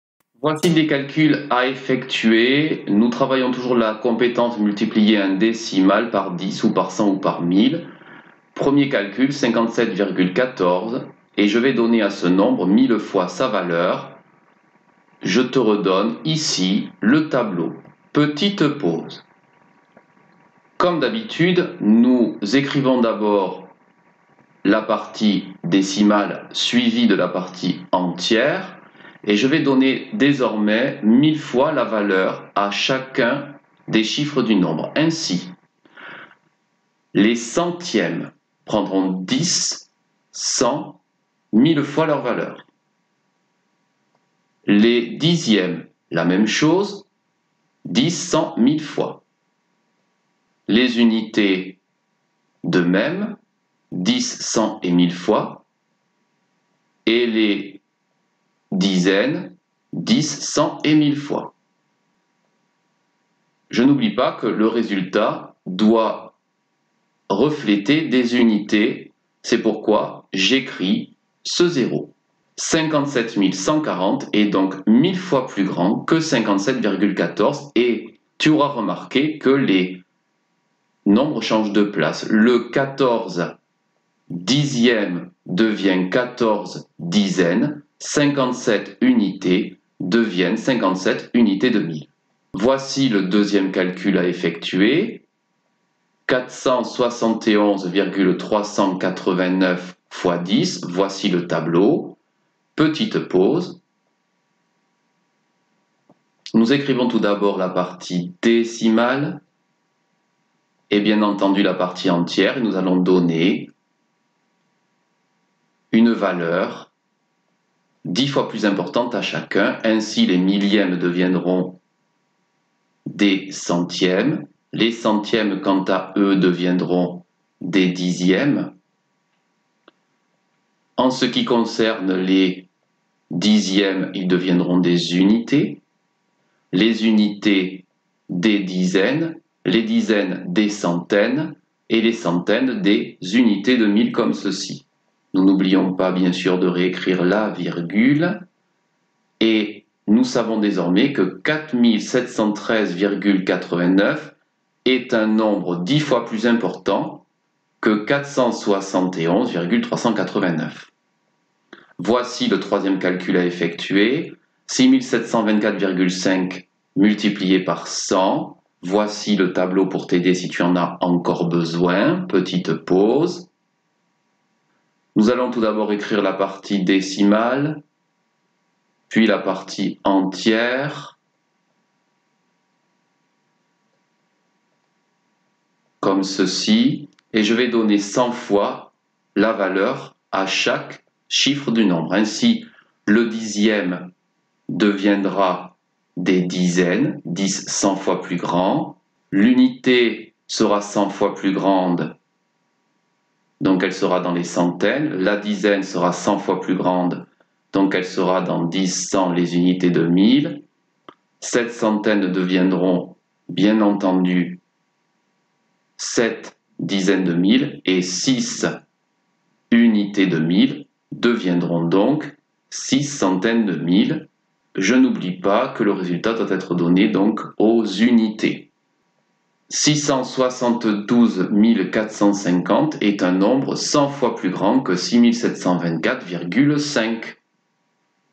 F2 Multiplier un décimal par 10 ou 100 ou 1 000
Dans cette vidéo, tu t'entraîneras à multiplier un nombre décimal par 10, par 100 ou par 1 000.